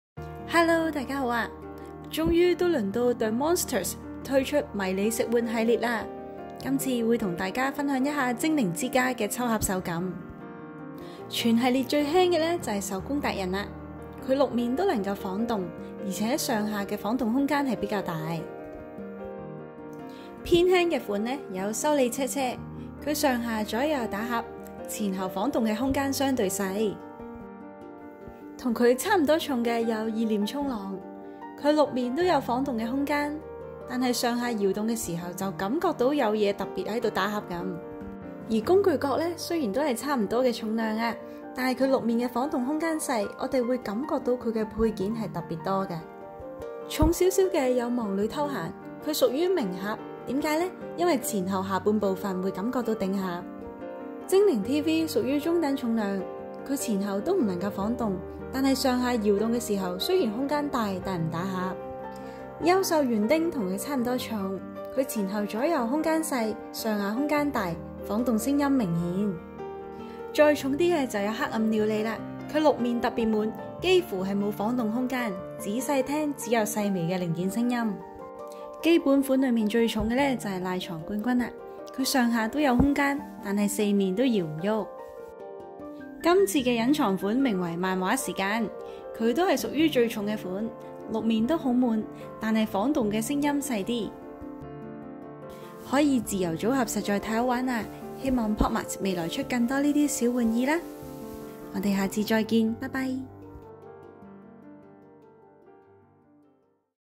Labubu 精靈之家糸列 The Monsters Home of the Elves PROP 全套連隱藏版 重量 手感 心得 分享【POP MART 泡泡瑪特 手辦道具 Mini Me】
終於到Labubu 推出迷你食玩了!!
可玩性一向的高呢！